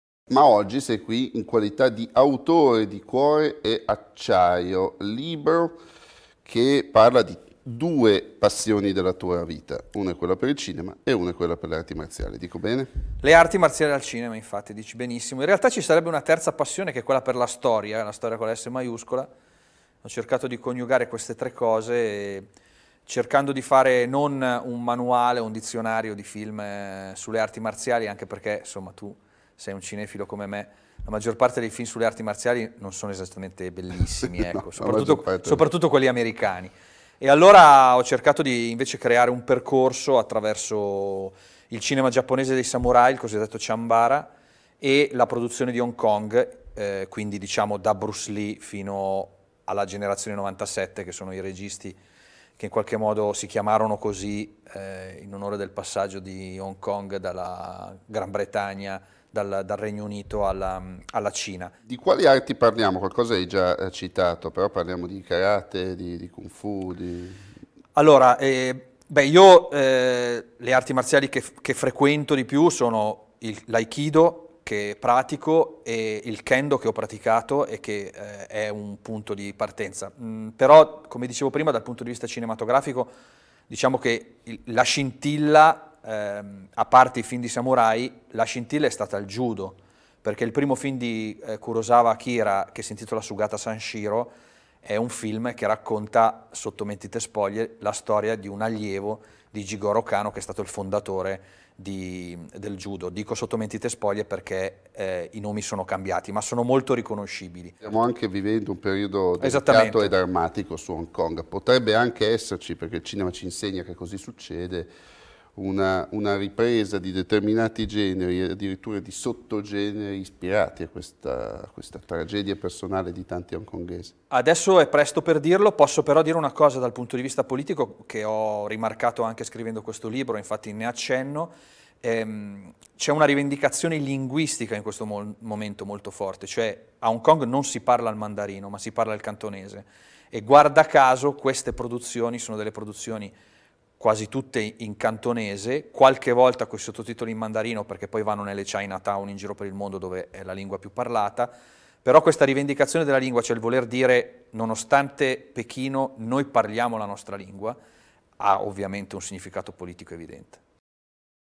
Non solo Hong Kong, le arti marziali al Cinema
Ospite della quarta puntata di CineAmando, il critico, giornalista e scrittore Mauro Gervasini presenta il "Cuore e Acciaio, le arti marziali al cinema", libro che mette insieme le sue tre grandi passioni: Settima Arte, Arti Marziali e Storia Contemporanea. Il volume spazia dalle produzioni giapponesi a quelle cinesi, da Hollywood alla Thailandia. E trova una delle sue più significative capitali artistiche in Hong Kong, dove spicca la cosiddetta "Generazione '97".